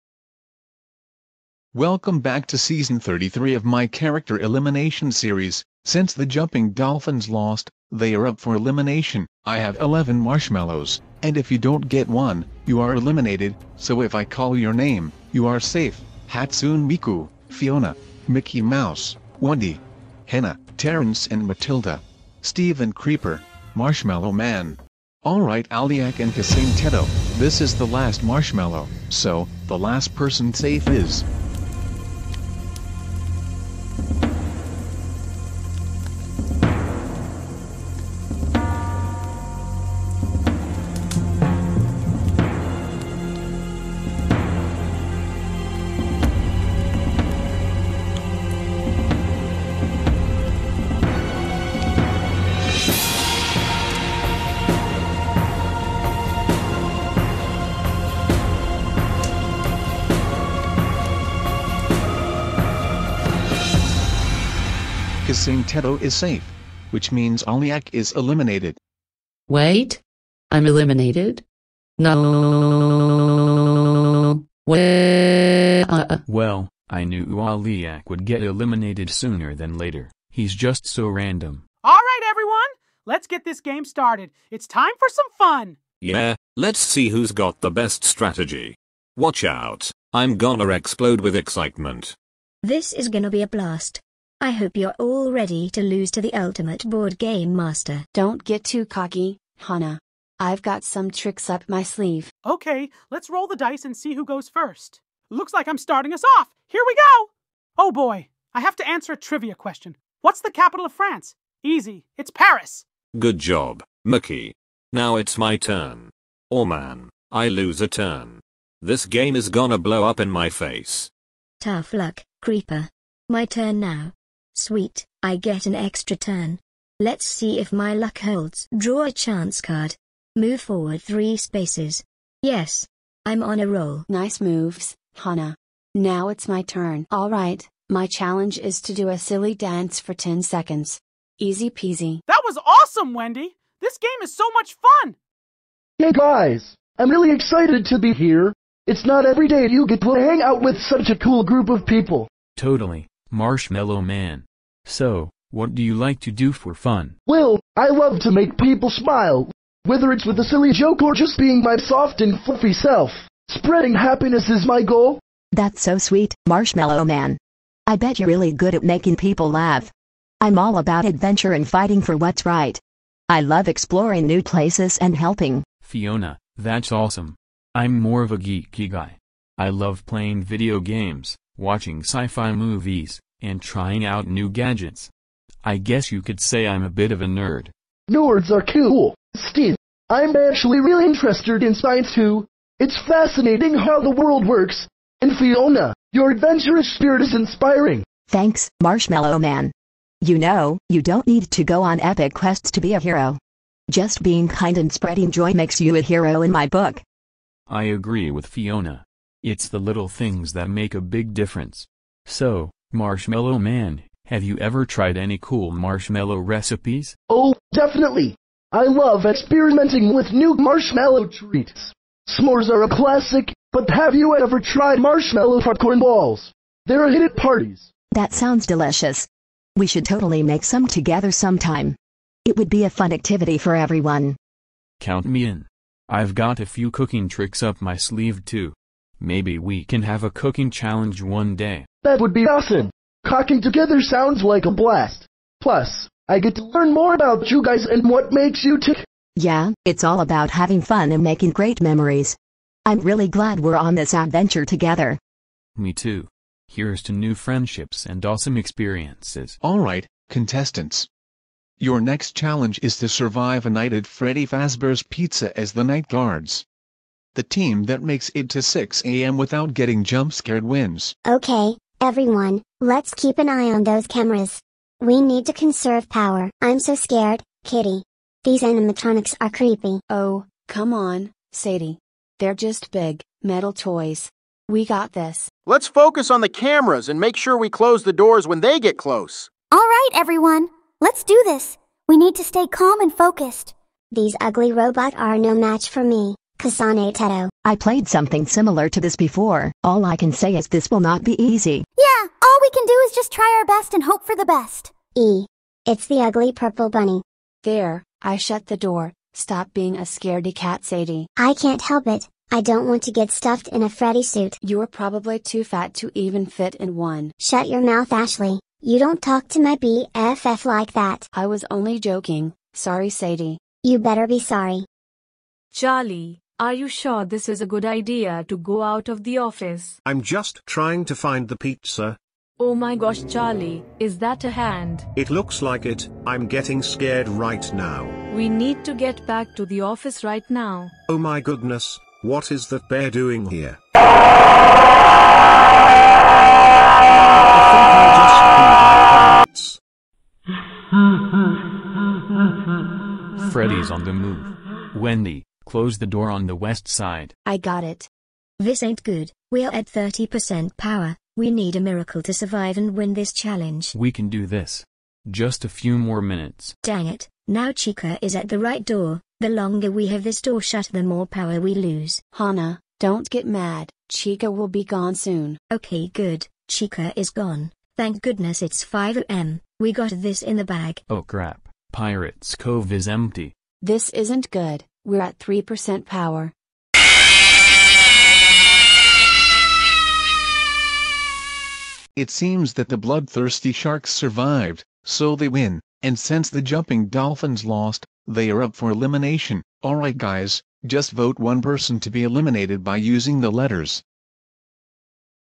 Character Elimination Season 33 Episode 3 1 Night at Freddy's!
In this Episode, the contestants must survive a night at Freddy Fazbear's pizza!